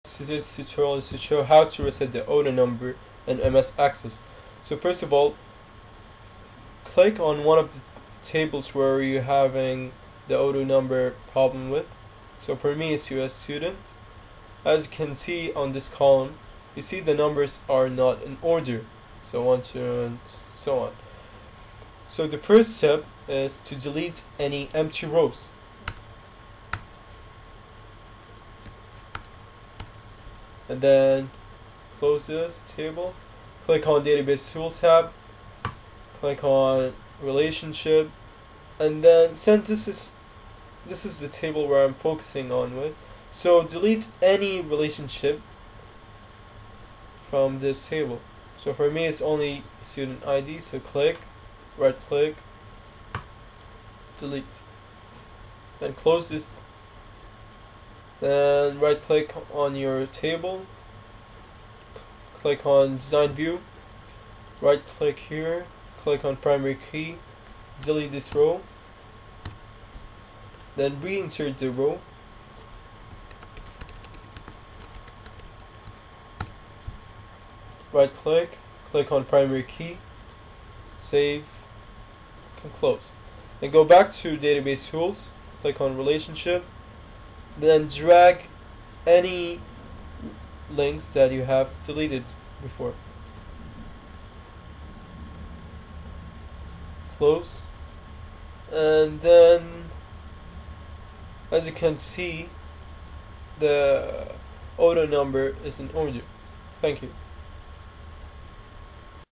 MS Access 2010/2013 - Reset AutoNumber
Simplest Tutorial showing you how to reset the auto number with few steps.